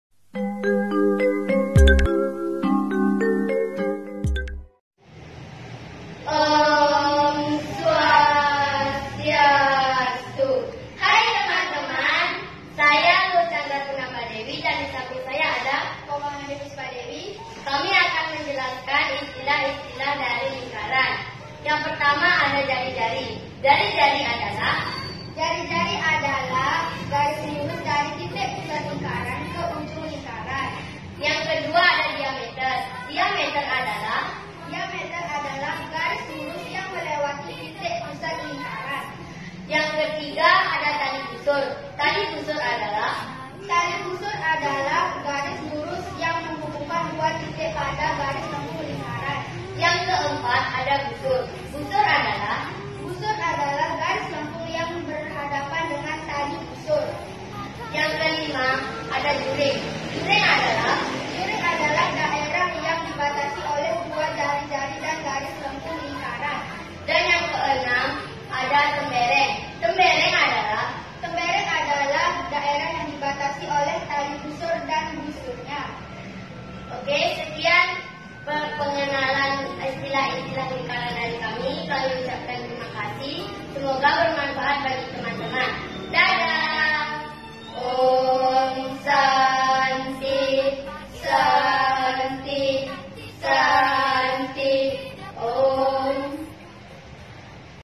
Istilah-istilah yang terdapat pada Lingkaran
Kelas : VI

Kompetensi Dasar
4.4 Peserta didik mampu mengidentifikasi titik pusat, jari-jari, diameter, busur, tali busur, tembereng, dan juring
Indikator
4.4.1 Mengidentifikasi istilah-istilah yang terdapat pada lingkaran yaitu titik pusat, jari-jari, diameter, busur, tali busur, tembereng, dan juring